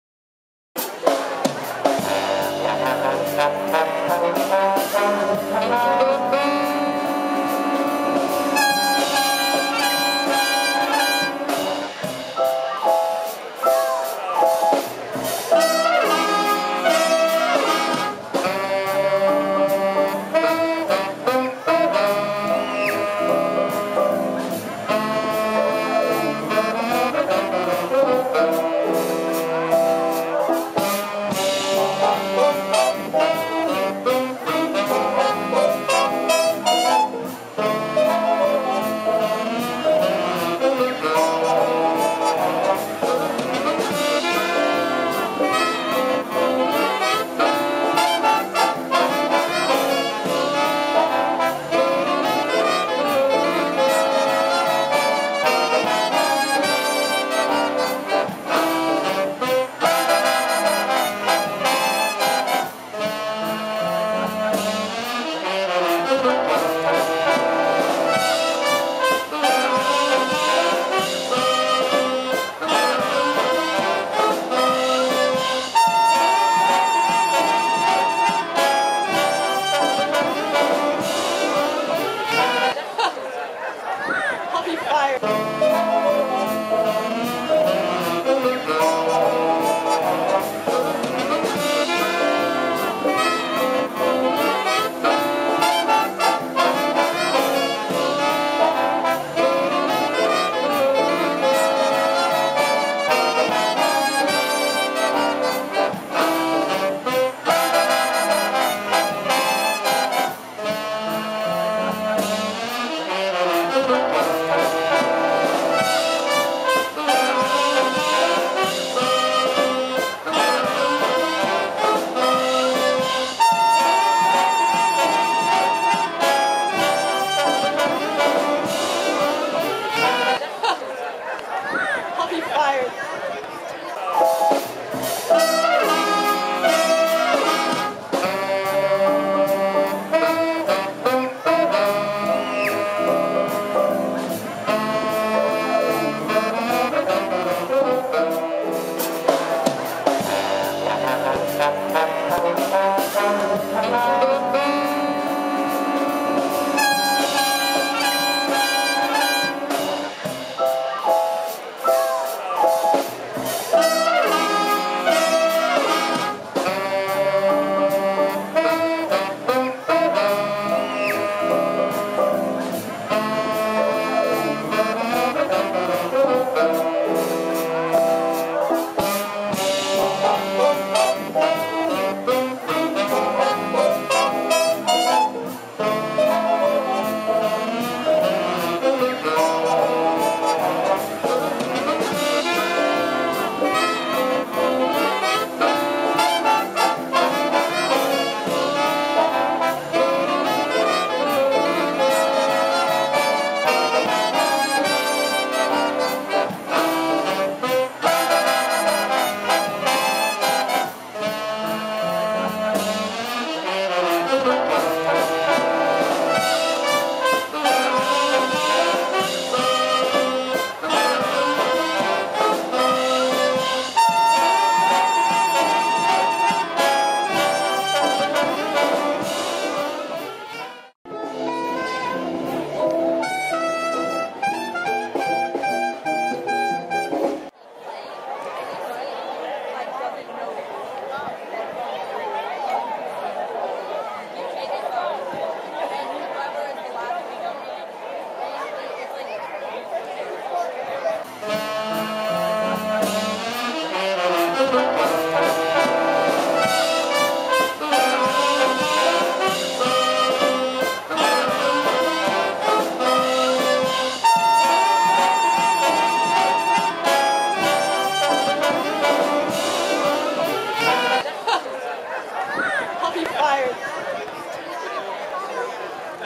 Concert in Portola Valley
An outdoor picnic and gathering of Portola Valley neighbors and friends in the Town Center Plaza on June 25, 2010.  The place was packed, not a parking space in sight!  The Concert  featured resident/vocalist Paige Fulkerson and the SF Bay Jazz Ensemble.  This was the first community event of its kind in the new Town Center Complex. The event was lots of fun and a resounding success!